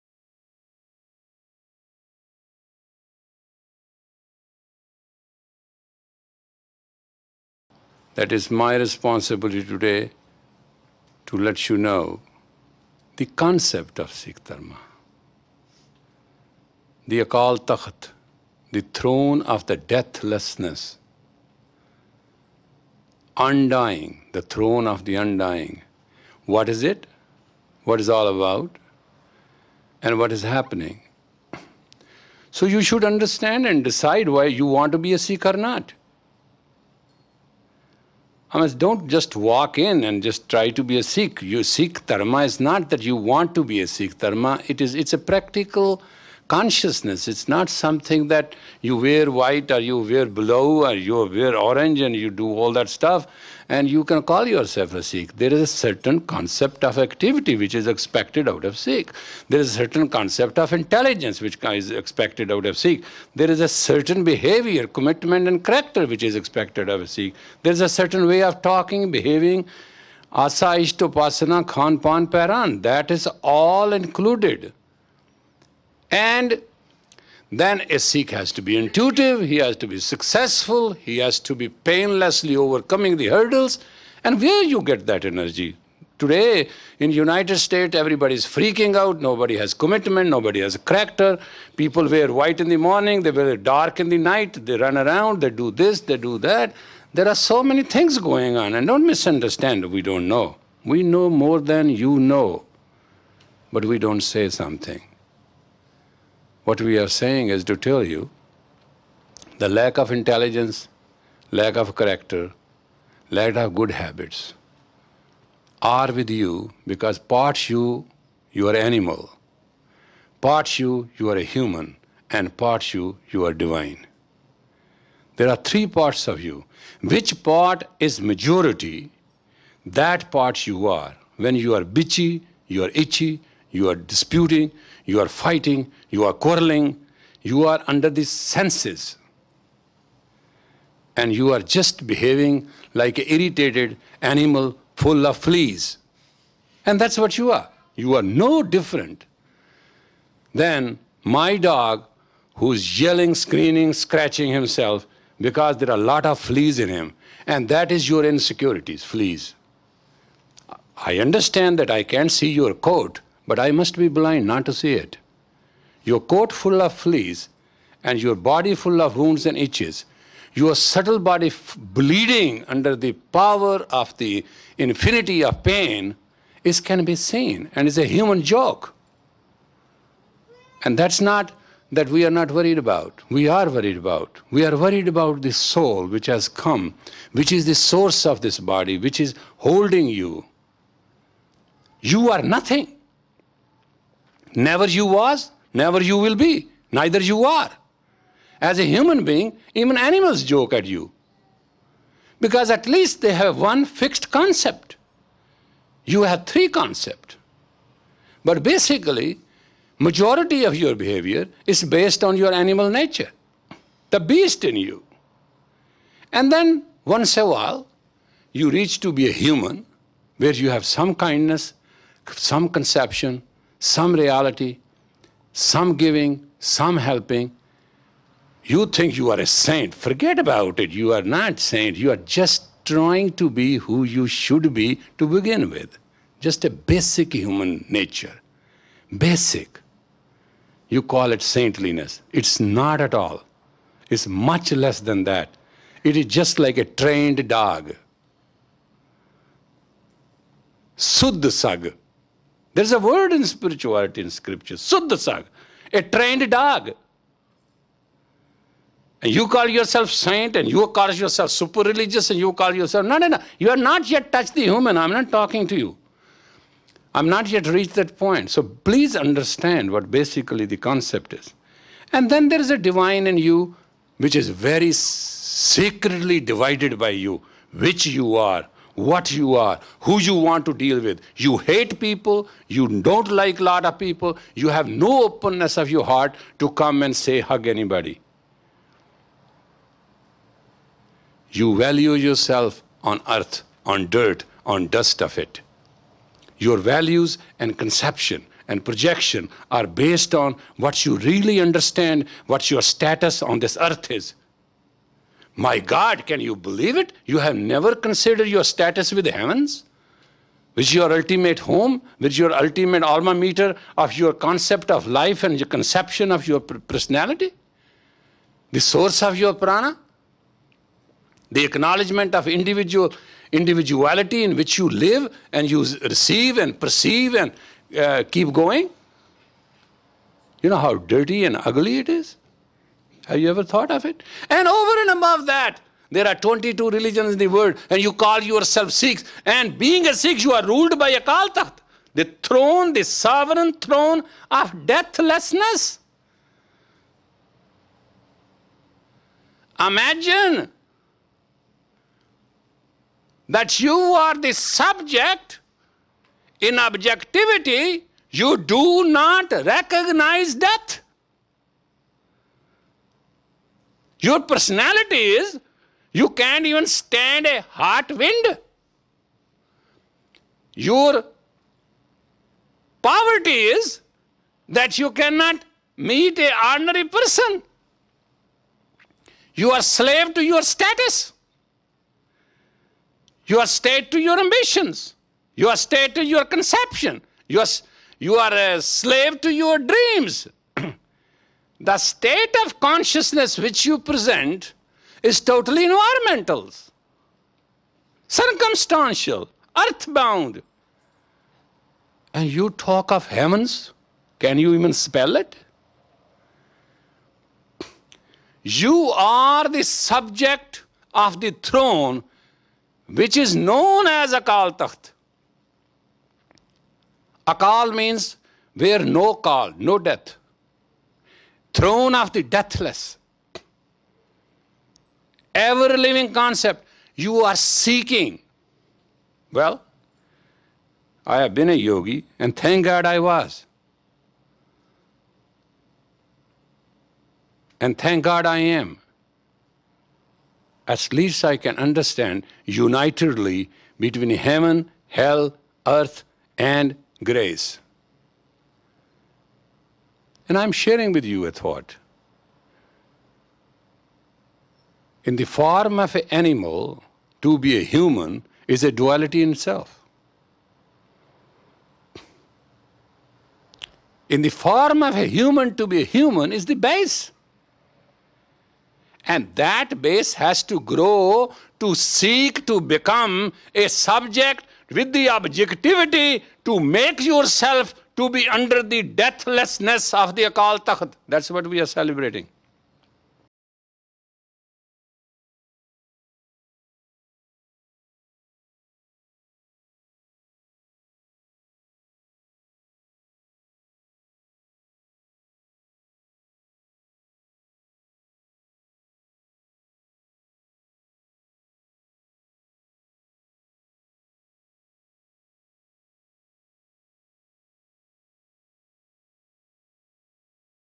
The Throne of Deathlessness (excerpt)
Guru Ram Das Ashram, Los Angeles, CA USA

On June 6, 1984, the Akal Takhat, the seat of religious authority for Sikhs, was attacked. Many remember the tanks and bullets and thousands of lives lost, yet through the lens of the unseen, we see God in everything. Like the phoenix that rises from the ashes, Khalsa will always prevail.

The Siri Singh Sahib believed it is an opportunity - a wake up call. Now is the time for Khalsa to unite in the spirit of the Undying, Infinite Truth embodied in the Akal Takht. In honor of all sacrifices that have paved this path we walk together, let this anniversary awaken the spirit of the Khalsa within you!

Sikh Dharma International invites you to celebrate this awakening with us. In this historic video Gurdwara lectures by the Siri Singh Sahib, he explains the significance of this event in depth and how it relates to Sikhs and the whole of humanity. Join us in this journey of awakening in this season of renewal.